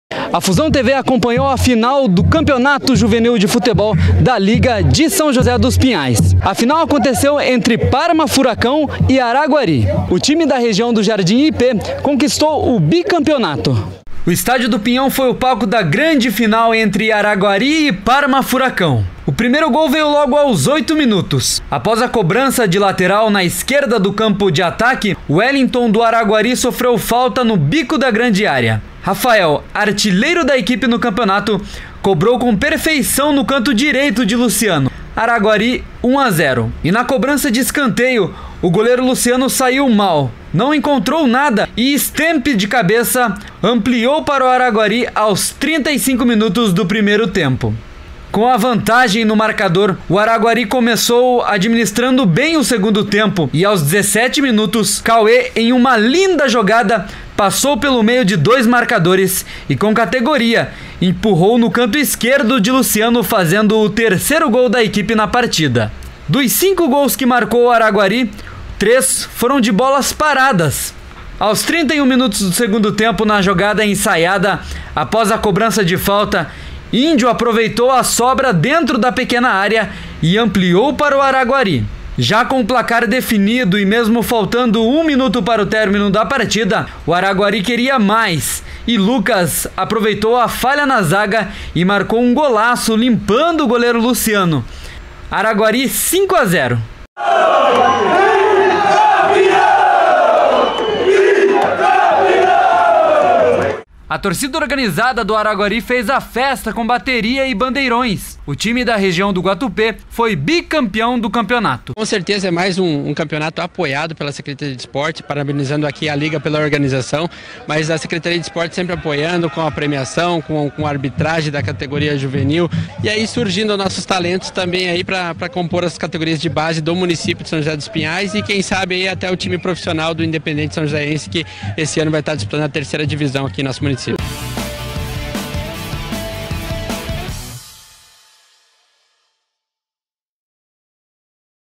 VT FINAL LIGA DE FUTEBOL DE SJP SUB-17
Com festa da torcida e goleada, Araguari F.C é Bi-Campeão do Campeonato Juvenil da Liga de São José dos Pinhais. No Estádio do Pinhão, o time da região do Guatupê venceu o Parma/Furacão pelo marcador de 5x0.